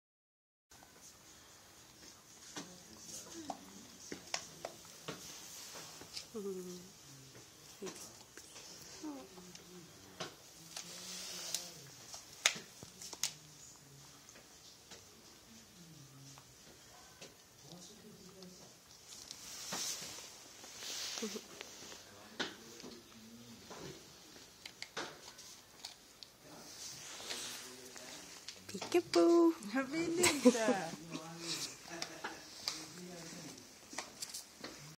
Bess- eurasian dog with her chewtoy
Bess- eurasian dog with her chewtoy. Cute like a cuddly toy. :)